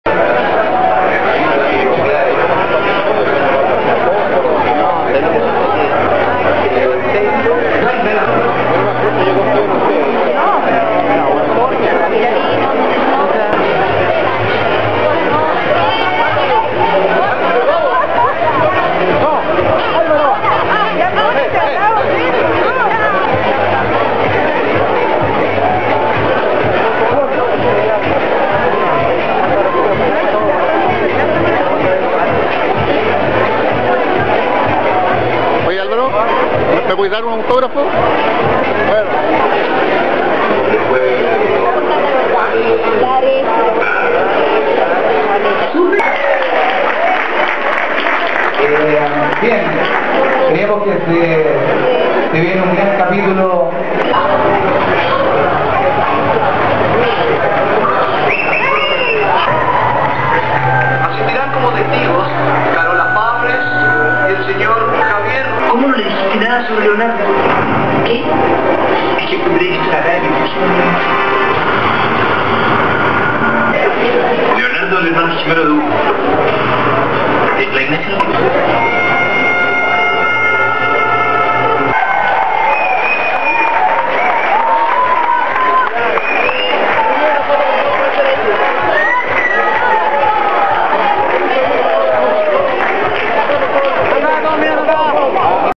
La teleserie de Le�n
La semana que termina fue el lanzamiento de "La familia de al lado", la nueva apuesta de TVN, y Le�n no pod�a estar ausente de este evento que se efectu� en una especie de lobby en TVN, con actores, periodistas y ejecutivos. �lvaro Rudolphy le dio un aut�grafo a nuestro cronista, Jaime Vadell convers� sobre el futuro de este pasqu�n y Mauro Vald�s, el nuevo director ejecutivo, manifest� su cari�o por Delfina Guzm�n. Hubo �sco sour, mucha champa�a y "Pi�eras", el nuevo s�ndwich que Confiter�a Torres patent� hace poco.